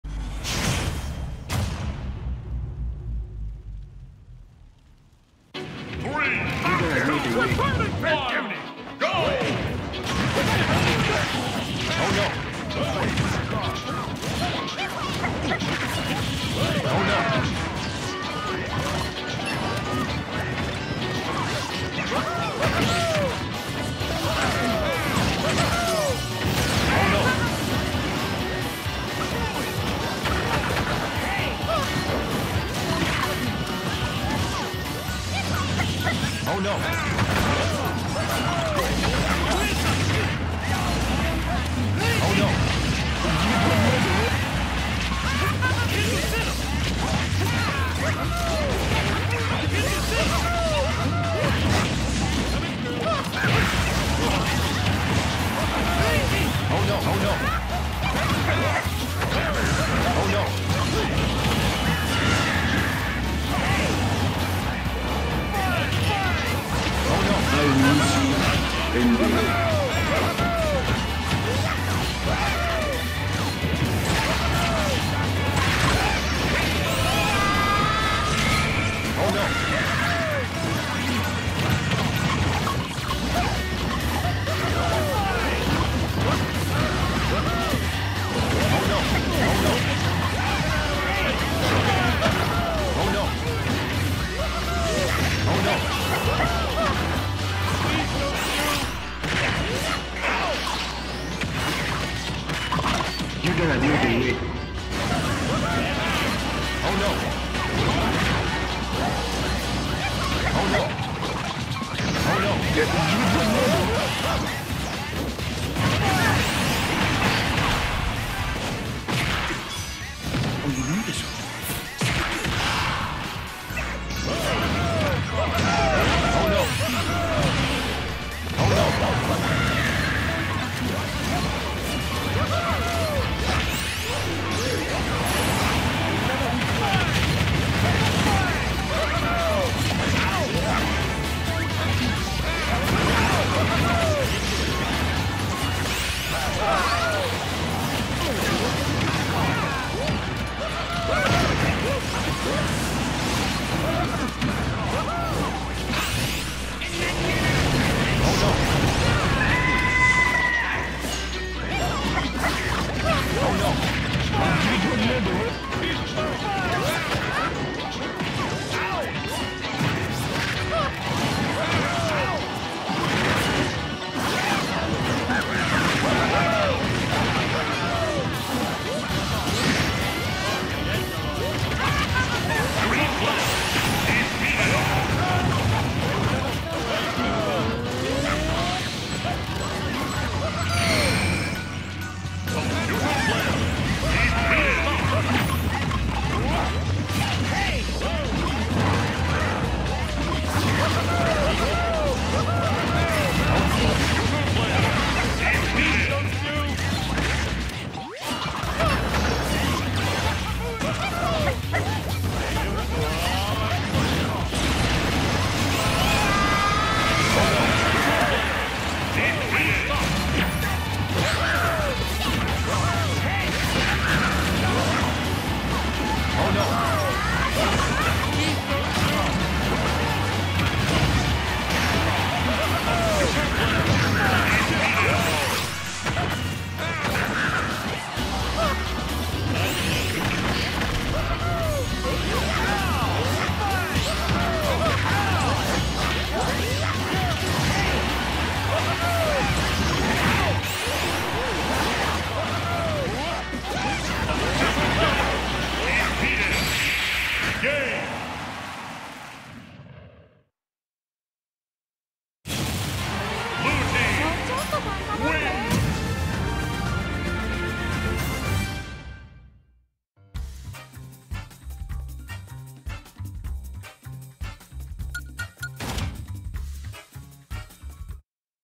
Dry Bowser, Ruby Rose, Bonkers & Shrek vs Ugandan Knuckles, Miku, Po & Homer
Starting things off today is a 4 v 4 team battle.

Dark Dry Bowser, Ruby Rose, Bonkers and Shrek vs Ugandan Knuckles, Miku, Po and Homer Simpson.